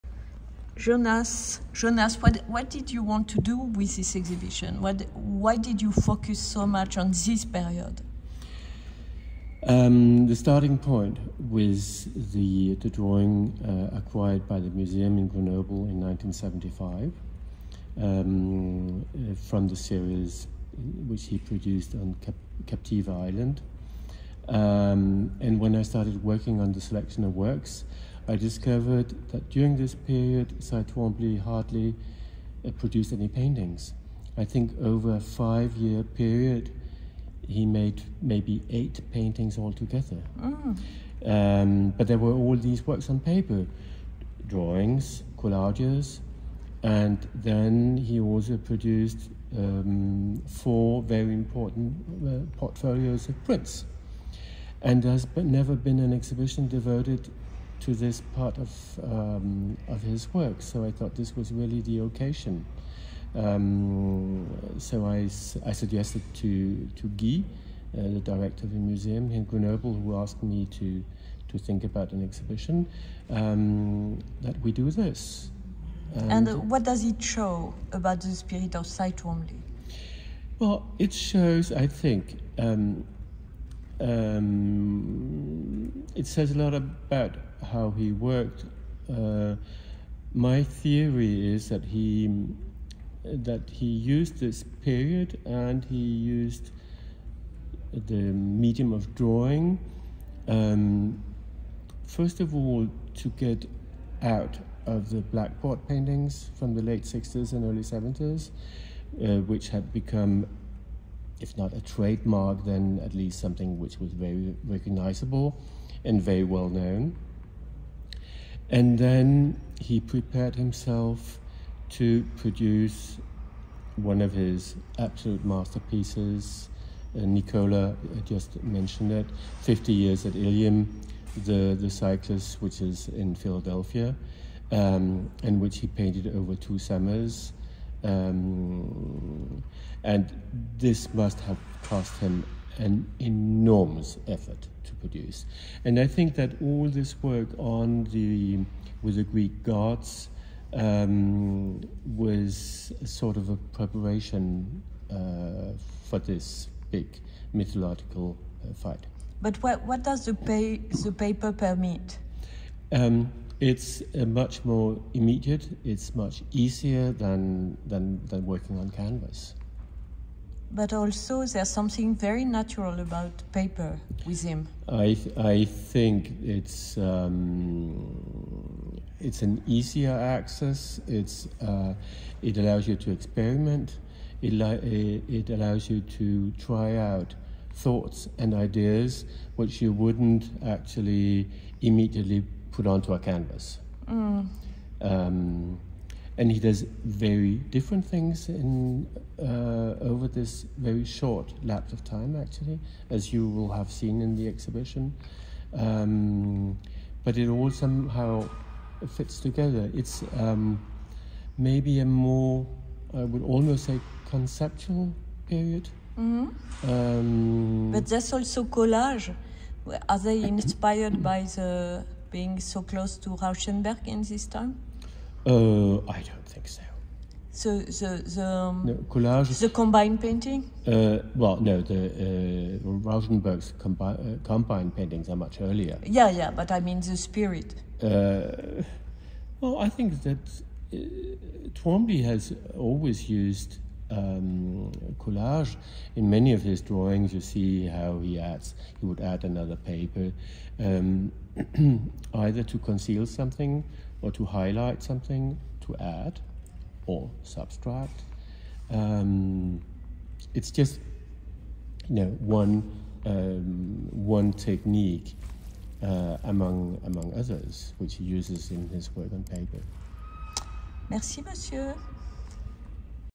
Jonas Storsve, Co Curator, Cy Twombly works on paper (1973-1977), Musée de Grenoble, June 2023
Read the report: "Cy Twombly: some secrets revealed about the gestation of his works"